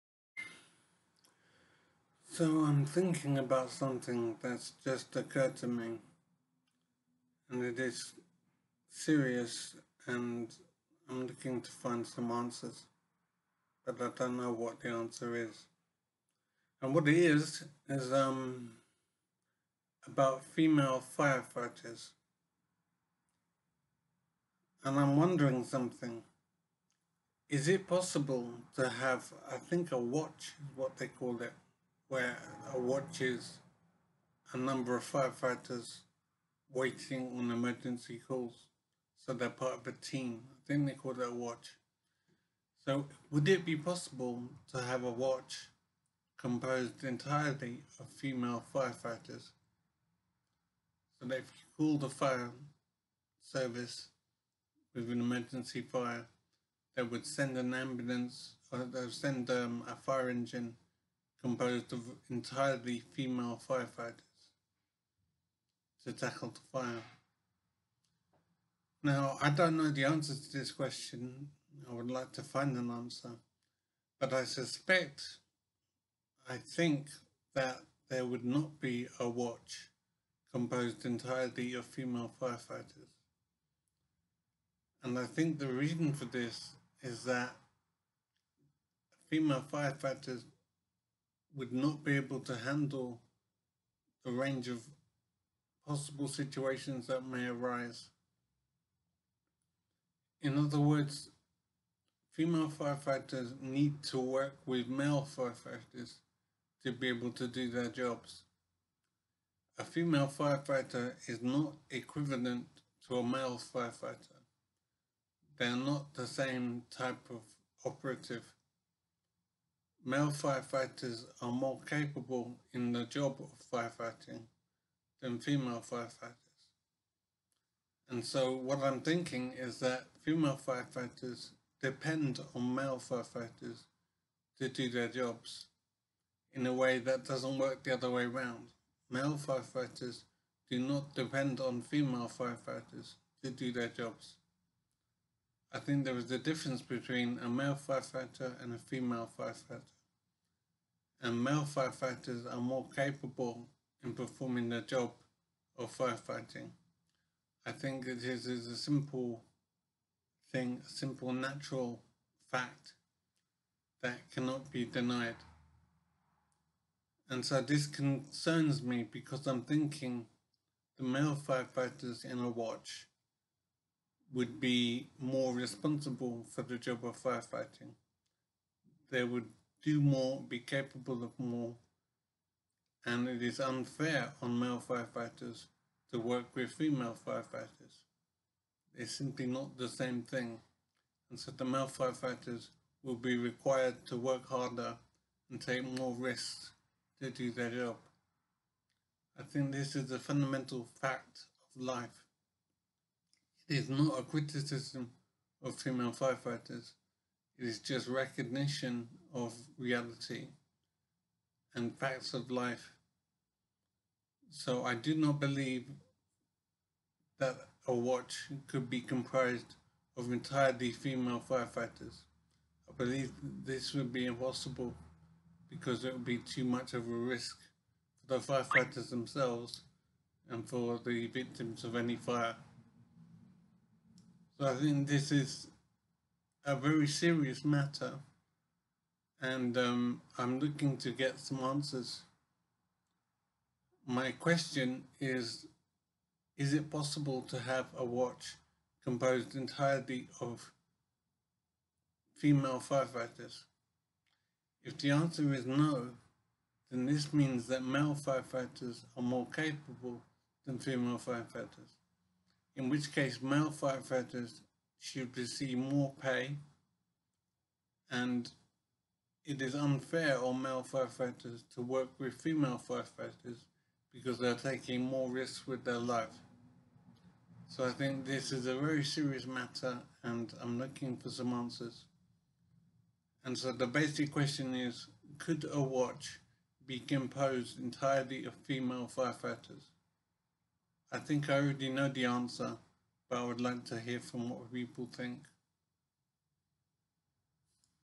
Female firefighters are not as capable, as male firefighters?
Do female firefighters do the same job and take the same risks as male firefighters?

I think that they do not and that makes this question very serious and possibly life-endangering!